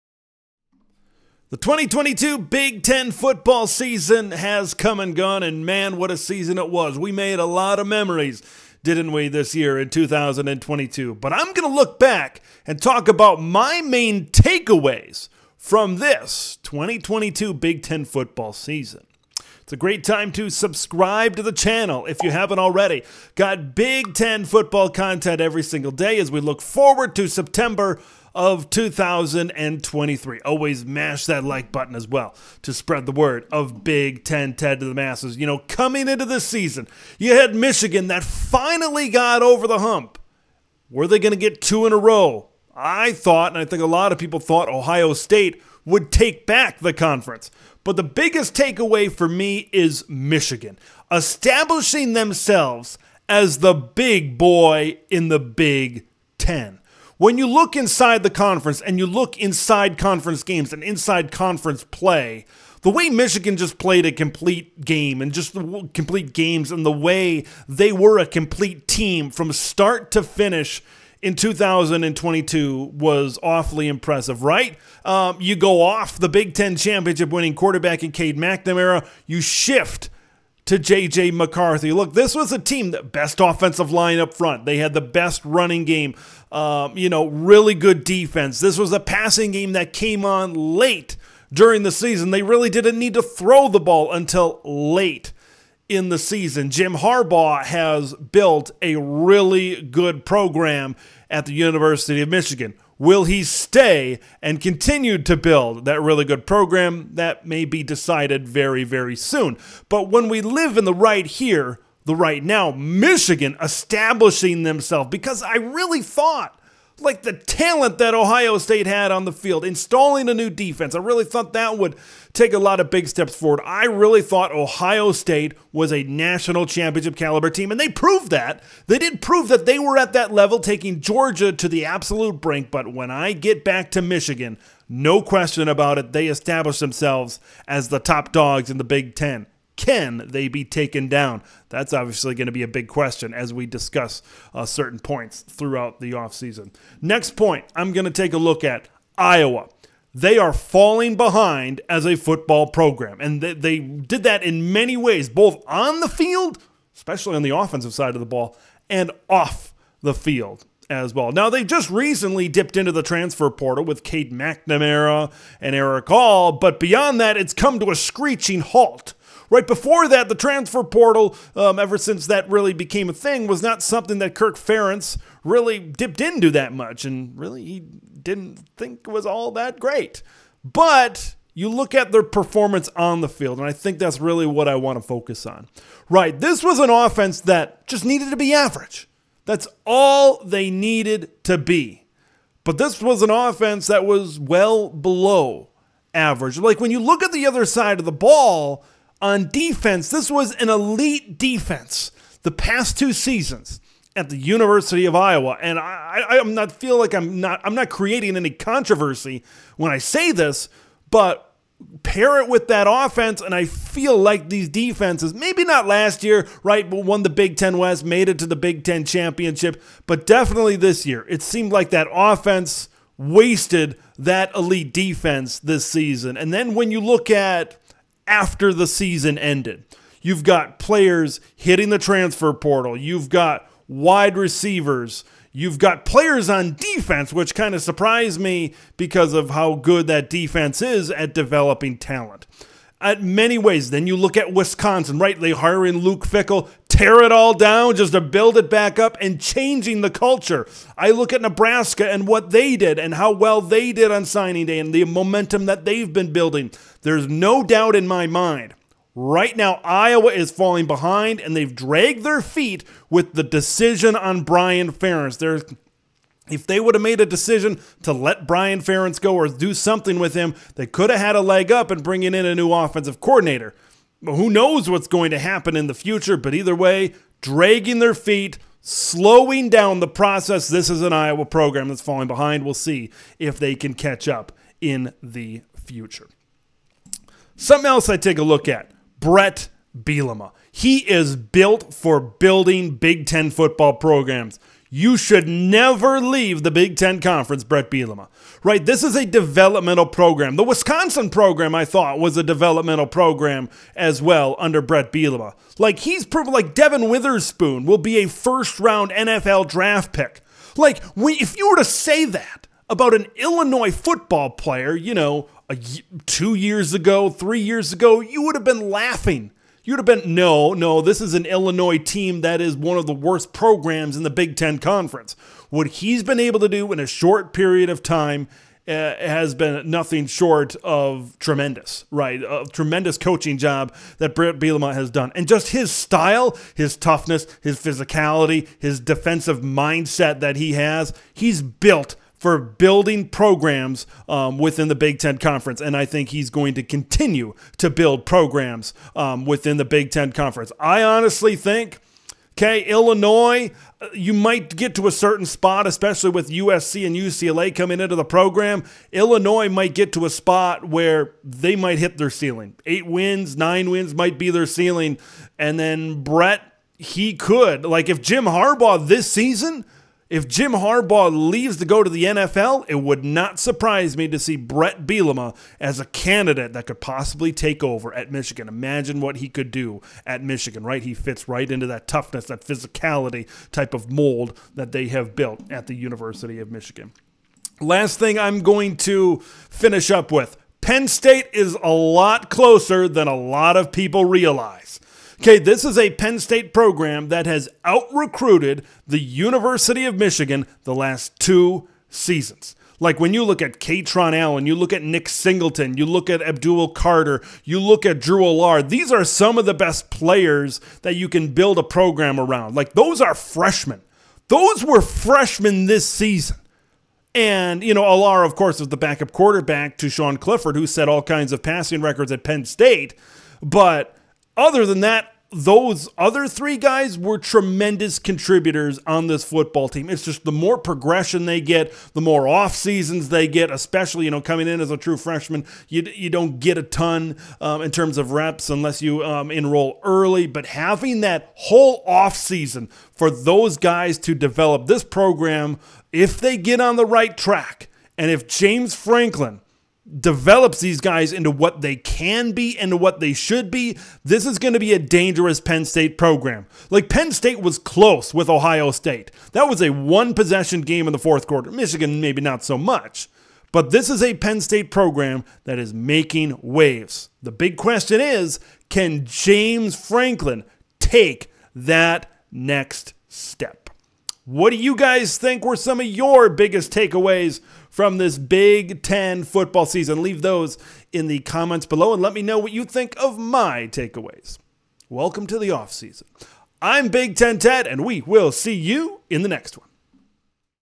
Takeaways from the 2022 Big Ten Football Season : Michigan the B1G Boys, Iowa Falling Behind!
The 2022 Big Ten football season has come and gone, and with it lots of memories to look back on. I am talking about my takeaways from the Big Ten season.